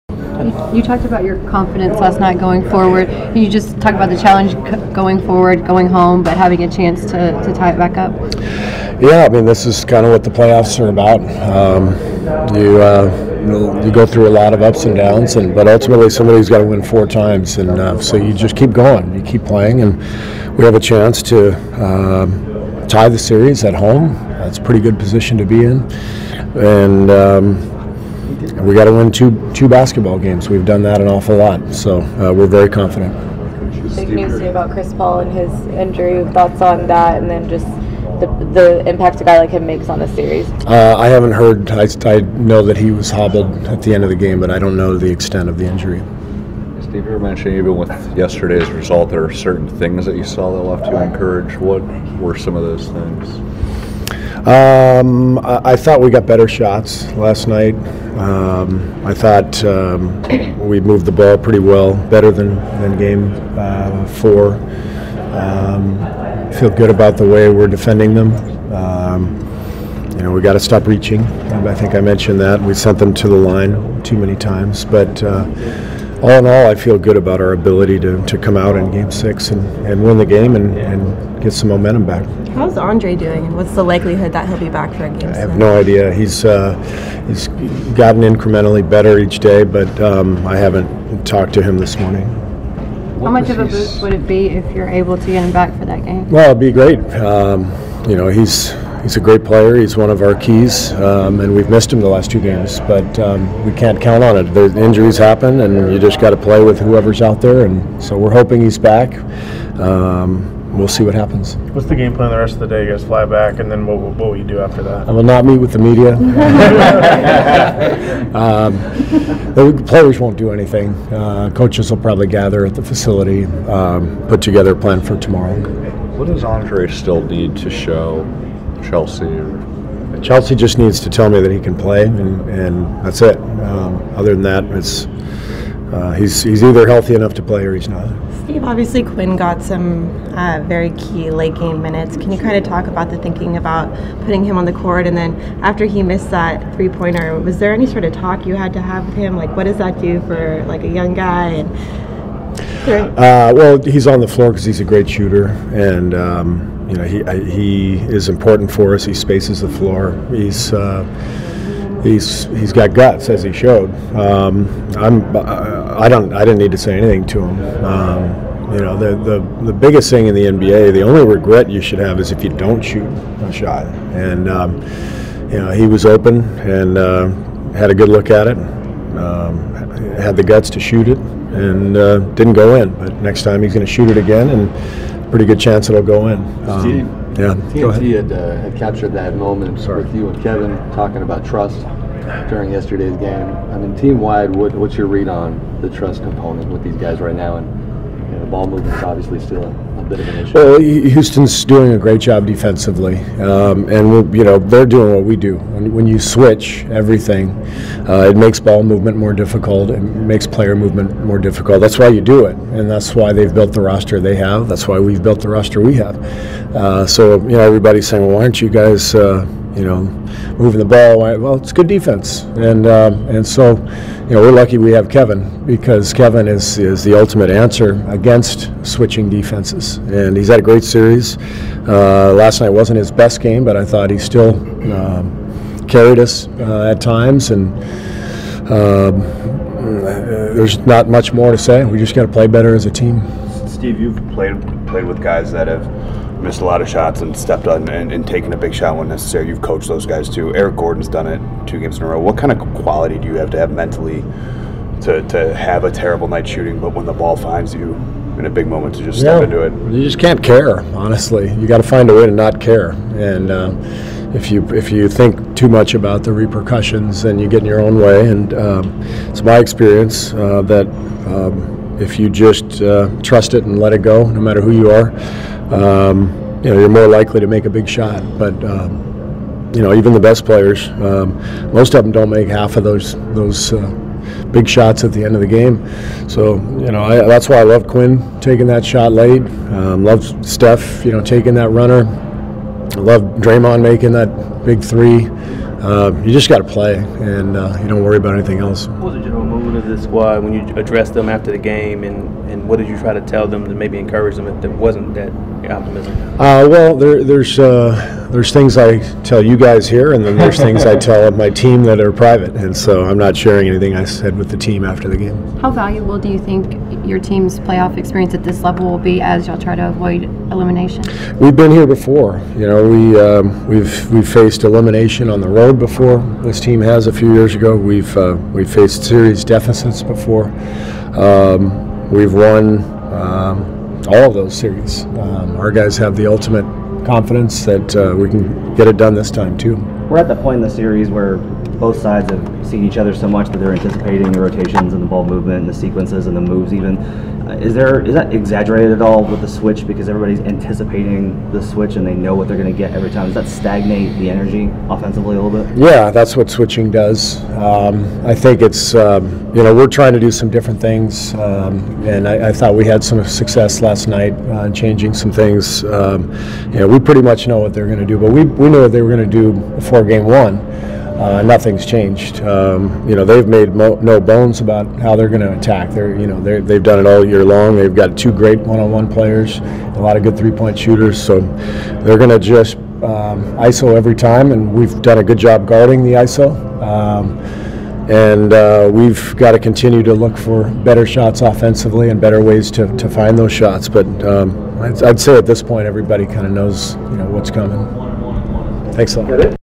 Entire STEVE KERR availability: Iguodala status, confidence for Game 6, playing "iso" ball
...from Warriors media availability at team hotel in Houston, day before 2018 Western Conference Finals, Game 6.